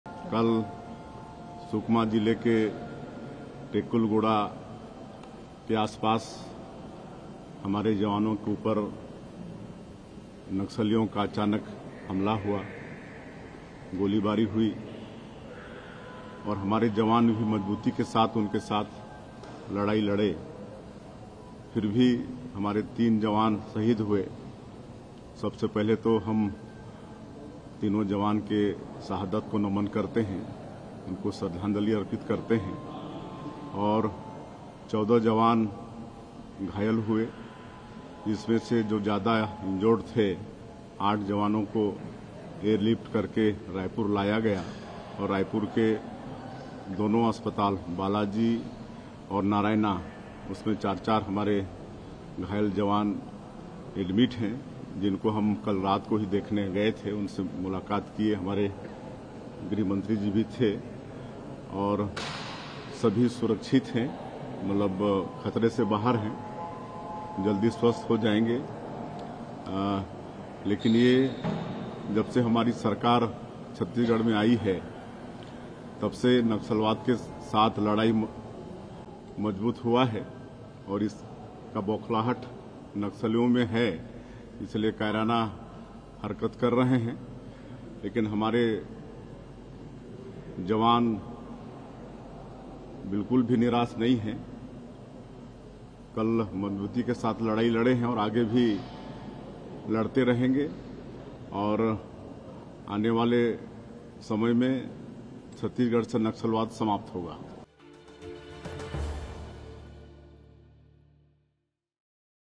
'Naxalism will end in Chhattisgarh…': CM Vishnu Deo Sai on encounter near Bijapur-Sukma border
Raipur (Chhattisgarh): Speaking on the encounter with Naxals near Bijapur-Sukma border, Chhattisgarh CM Vishnu Deo Sai on January 31 said that Naxalism will end in Chhattisgarh. Vishnu Deo Sai said, “Ever since our government came to power in Chhattisgarh, the fight against Naxalism has strengthened...Our soldiers fought with strength yesterday and will continue to fight in the future also. In the coming times, Naxalism will end in Chhattisgarh.” Three personnel of the Central Reserve Police Force (CRPF) were killed and 14 others injured in the exchange of fire with Naxals in Chhattisgarh's Bijapur district on January 30. The incident occurred near Tekalgudem village when a joint team of security personnel was out on a search operation.